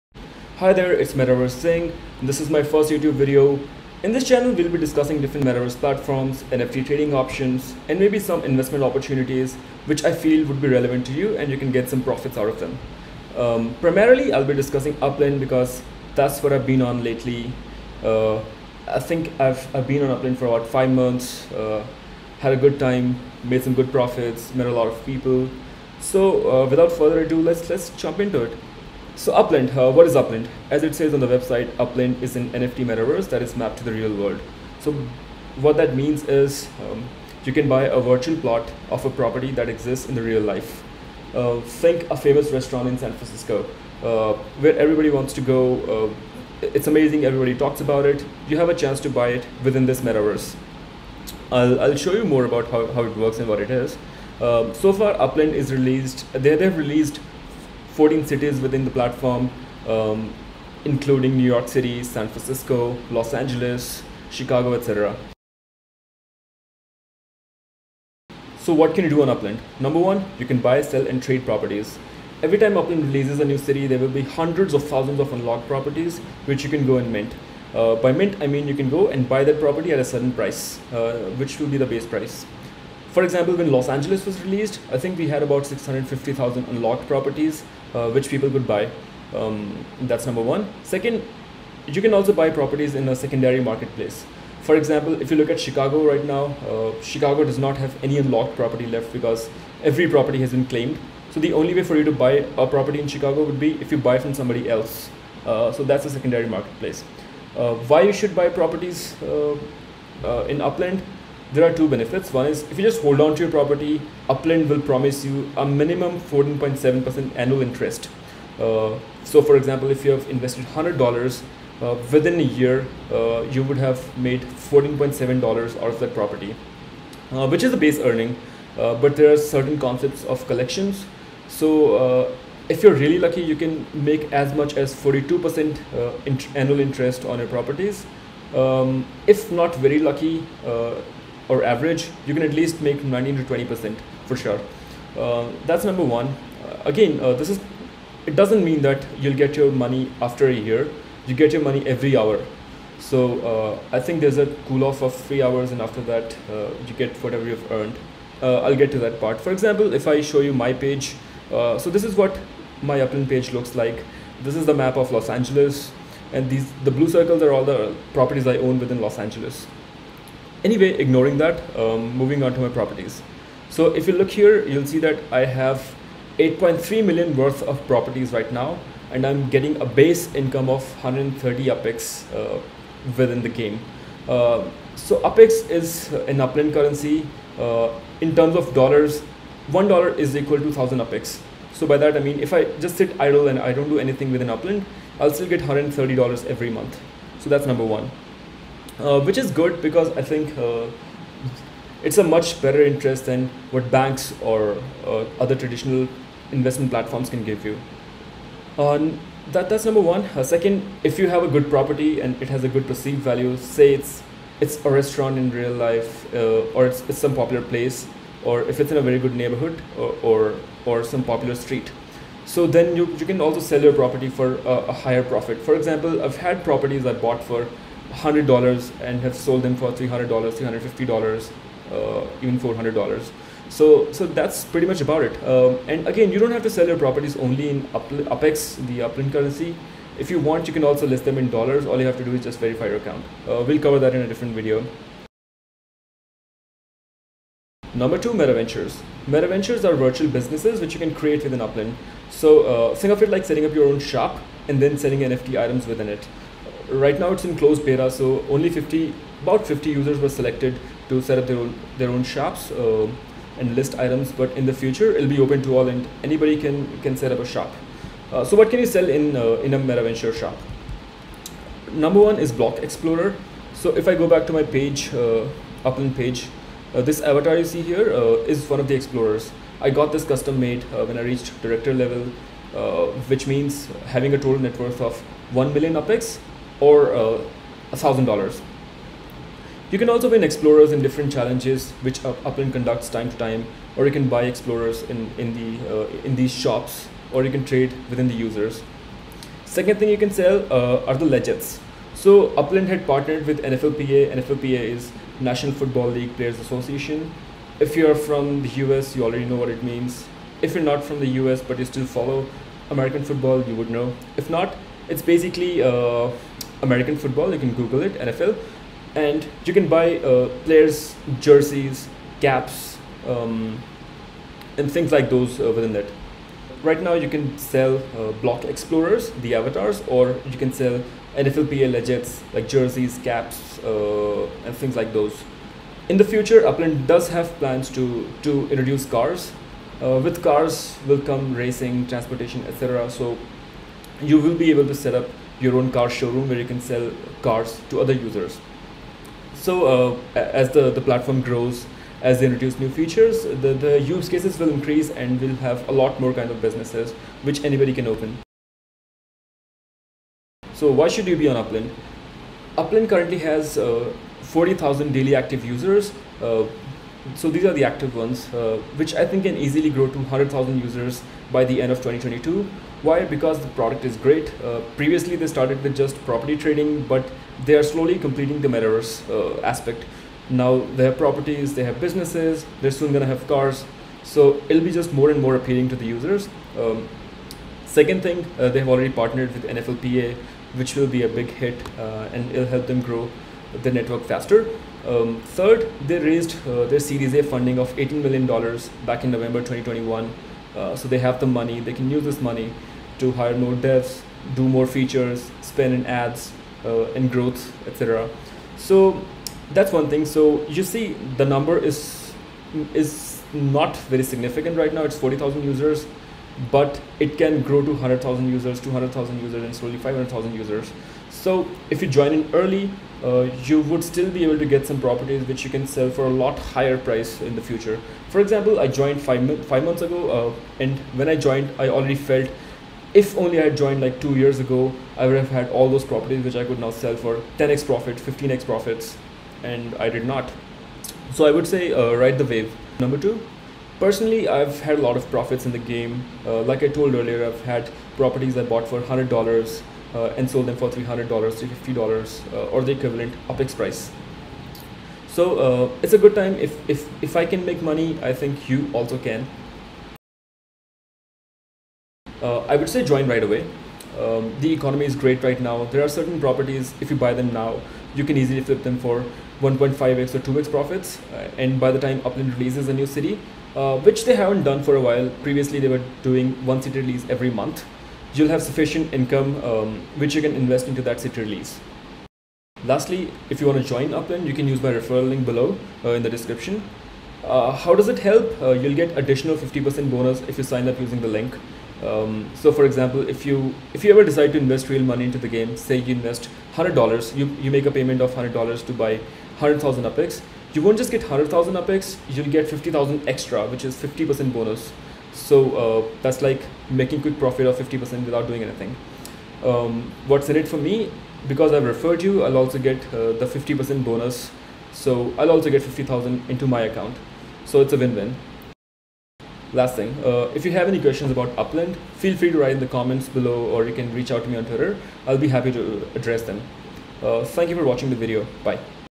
What is Upland? (Learn more about NFT Property Trading in Metaverse)
Learn more about NFT Property Trading in Upland

Upland is an NFT metaverse mapped to the real world. You get to own a virtual plot of a property that exists in the real world.

The video covers:
1. What is Upland?
2. What can you do on Upland?
3. Why should you be on Upland?
4. When is a good time to join?